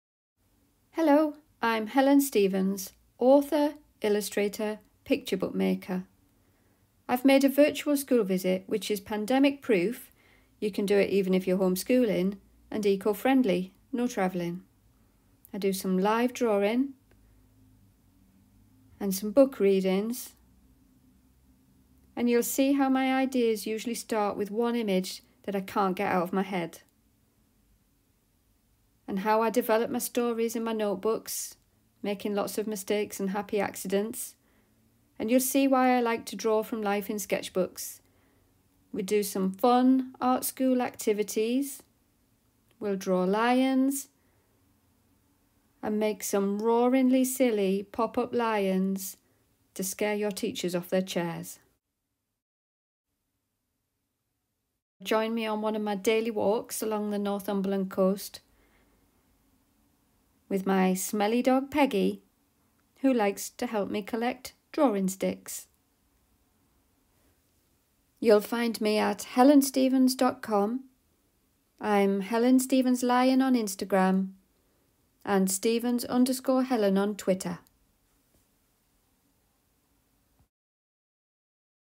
Helen Stephens - Virtual School Vists
Helen Stephens, author, illustrator, picture book maker

Classic Virtual School Visits Package, available from my website.

WHAT’S INCLUDED:

A series of fun videos where I chat about how I make my books, a behind the scenes tour of my studio, a look through my sketchbooks and I show where my book ideas start. You could watch one video each day, or go at your own pace

Book reading: How to Hide a Lion

Colouring in sheets

A nature hunt and a fun art school activity

You will get to meet my smelly dog Peggy, join me on my daily walk along the Northumberland coast and see my process. I will show that I make mistakes like everyone does, and how mistakes can even be valuable!

It will be easy to use, no tricky tech and no special materials required. All delivered to your inbox.